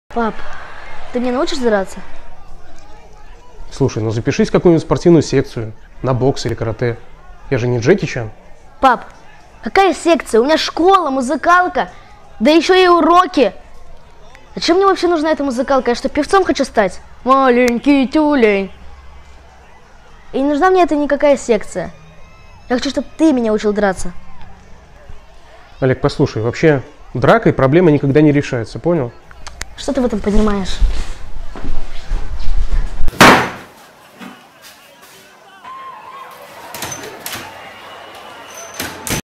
Научи меня драться ( продолжение, Никита Табунщик )
Большинство, посмотрев первую часть, предложило отправить ребенка в секцию каратэ или бокса. Вот как это получилось у наших героев...